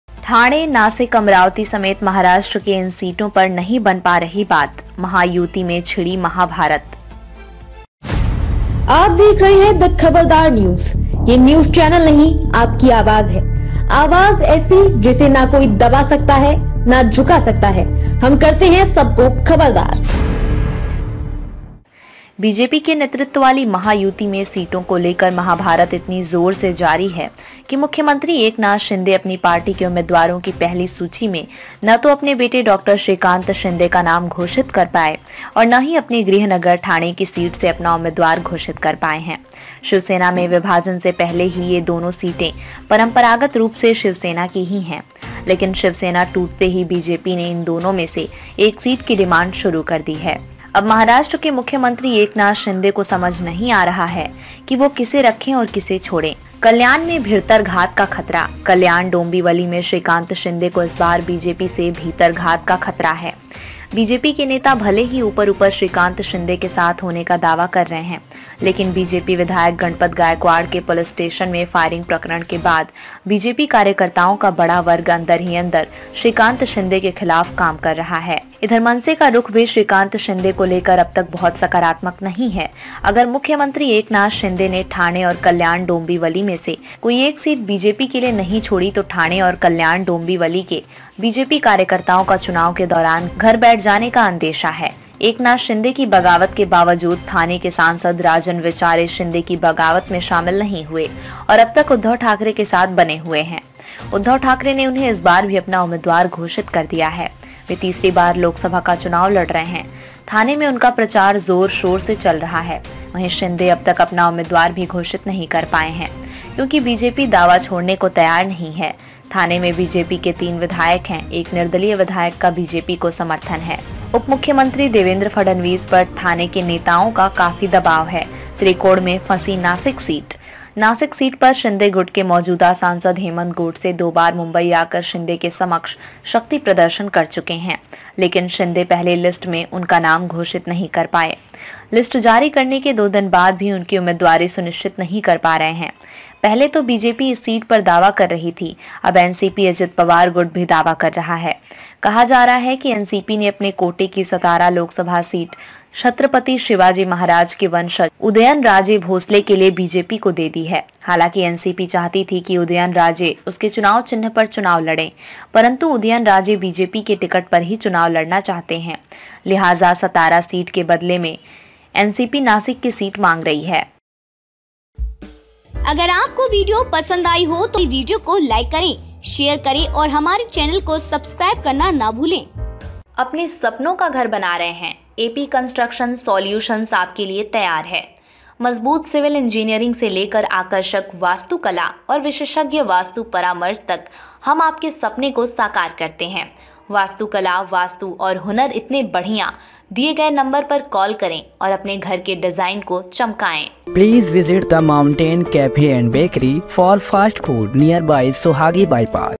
Thane, Nashik, Amravati, समेत Mahabharata की इन सीटो पर नहीं बन पा रही बात महायुति में छिड़ी महाभारत
ठाणे, नासिक, अमरावती समेत महाराष्ट्र की इन सीटों पर नहीं बन पा रही बात, महायुति में छिड़ी महाभारत
 बीजेपी के नेतृत्व वाली महायुति में सीटों को लेकर महाभारत इतनी जोर से जारी है कि मुख्यमंत्री एकनाथ शिंदे अपनी पार्टी के उम्मीदवारों की पहली सूची में न तो अपने बेटे डॉ. श्रीकांत शिंदे का नाम घोषित कर पाए और न ही अपने गृह नगर ठाणे की सीट से अपना उम्मीदवार घोषित कर पाए हैं। शिवसेना में विभाजन से पहले ही ये दोनों सीटें परंपरागत रूप से शिवसेना की ही हैं। लेकिन शिवसेना टूटते ही बीजेपी ने इन दोनों में से एक सीट की डिमांड शुरू कर दी है। अब महाराष्ट्र के मुख्यमंत्री एकनाथ शिंदे को समझ नहीं आ रहा कि वह किसे रखें और किसे छोड़े।कल्याण में भिरतरघात का खतरा
कल्याण-डोंबिवली में श्रीकांत शिंदे को इस बार बीजेपी से भितरघात का खतरा है। बीजेपी के नेता भले ही ऊपर-ऊपर श्रीकांत शिंदे के साथ होने का दावा कर रहे हैं, लेकिन बीजेपी विधायक गणपत गायकवाड के पुलिस स्टेशन में फायरिंग प्रकरण के बाद बीजेपी कार्यकर्ताओं का बड़ा वर्ग अंदर ही अंदर श्रीकांत शिंदे के खिलाफ काम कर रहा है। इधर मनसे का रुख भी श्रीकांत शिंदे को लेकर अब तक बहुत सकारात्मक नहीं है। अगर मुख्यमंत्री एकनाथ शिंदे ने ठाणे और कल्याण-डोंबिवली में से कोई एक सीट बीजेपी की लिए नहीं छोड़ी तो ठाणे और कल्याण-डोंबिवली के बीजेपी कार्यकर्ताओं का चुनाव के दौरान घर बैठ जाने का अंदेशा है।अगर ठाणे की बात करें तो वह शिवसेना का मुंबई से भी पुराना गढ़ है। एकनाथ शिंदे की बगावत के बावजूद ठाणे के सांसद राजन विचारे शिंदे की बगावत में शामिल नहीं हुए और अब तक उद्धव ठाकरे के साथ बने हुए हैं। उद्धव ठाकरे ने उन्हें इस बार भी अपना उम्मीदवार घोषित कर दिया है। वे तीसरी बार लोकसभा का चुनाव लड़ रहे हैं। ठाणे में उनका प्रचार जोर-शोर से चल रहा है। वहीं, शिंदे अब तक अपना उम्मीदवार भी घोषित नहीं कर पाए हैं, क्योंकि बीजेपी दावा छोड़ने को तैयार नहीं है। ठाणे में बीजेपी के तीन विधायक हैं। एक निर्दलीय विधायक का बीजेपी को समर्थन है। उप मुख्यमंत्री देवेंद्र फडणवीस पर ठाणे के नेताओं का काफी दबाव है।